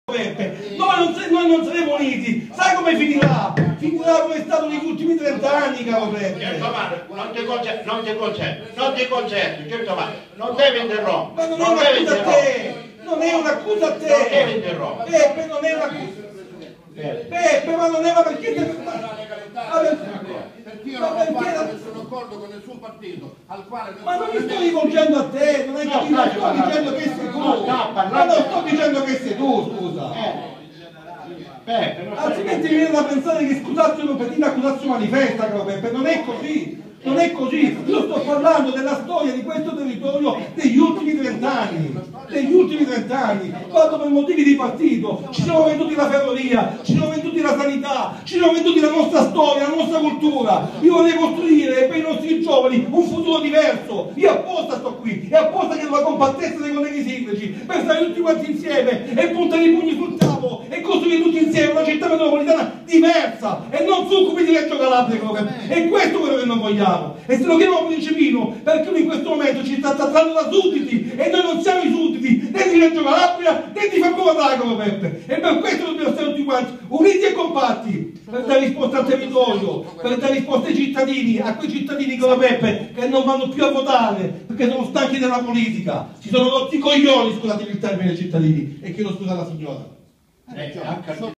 Giovanni Calabrese sulla città metropolitana
Assemblea dei sindaci della Locride del 26 febbraio 201